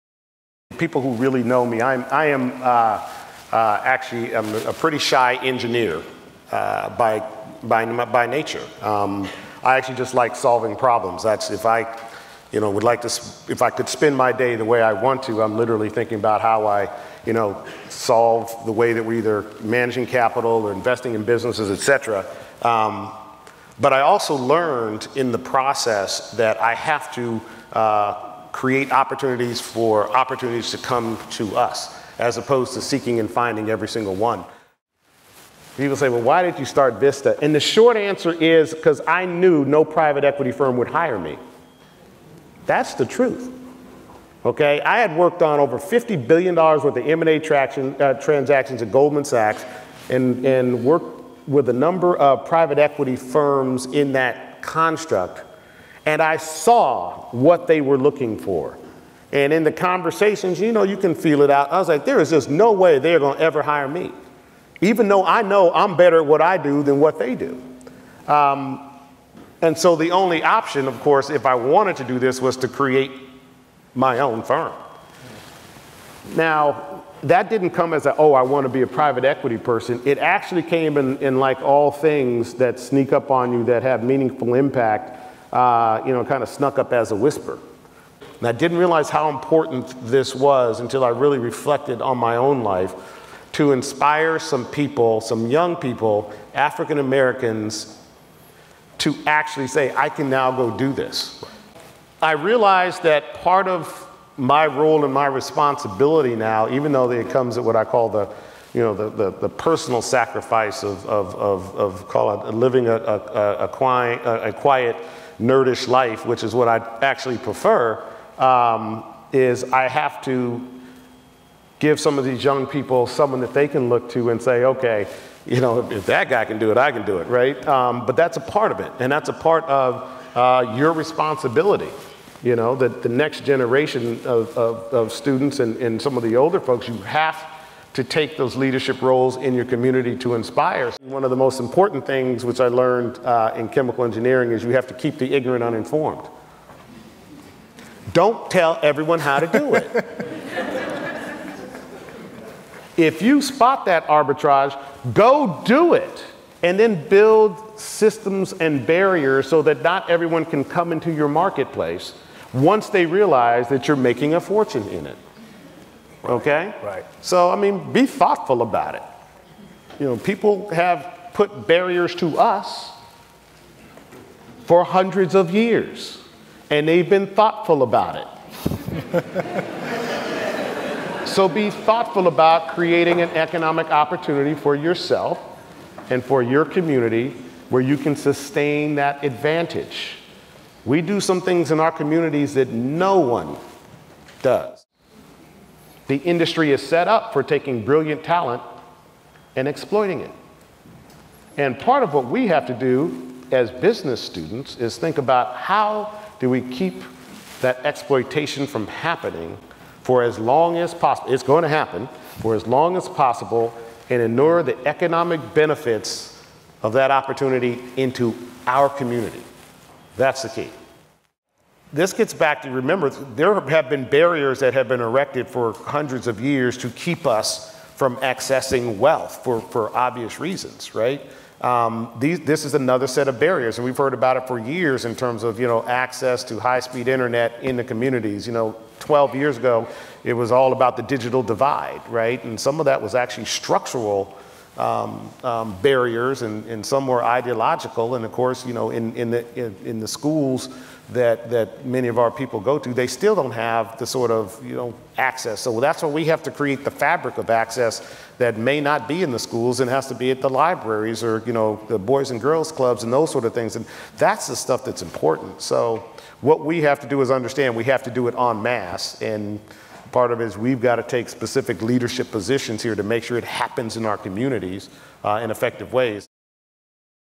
Billionaire Robert F. Smith on Community Investment
Robert F. Smith founder of Vista Equity Partners talks to the students of Colombia Business School's Black Business Students Association about strategies for personal finance and community development.